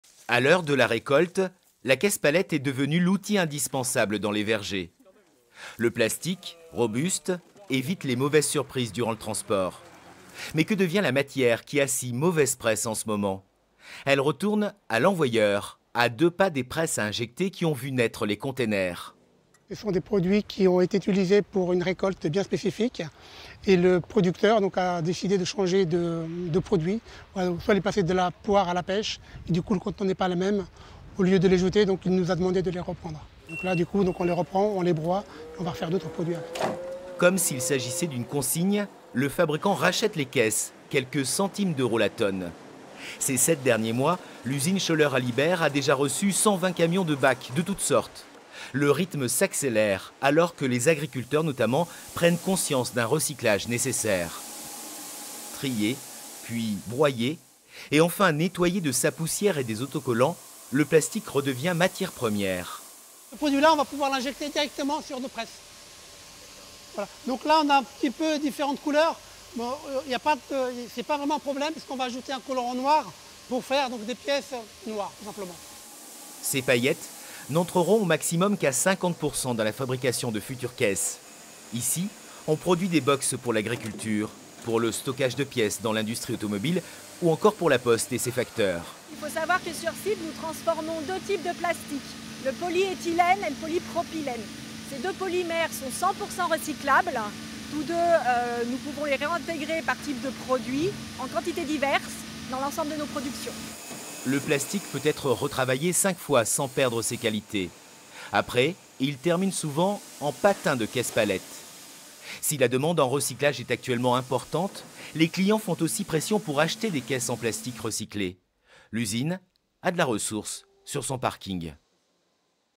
recyclage du plastique ,schoeller allibert nurieux
présentation du recyclage du plastique  , présenté par france 3 rhone alpes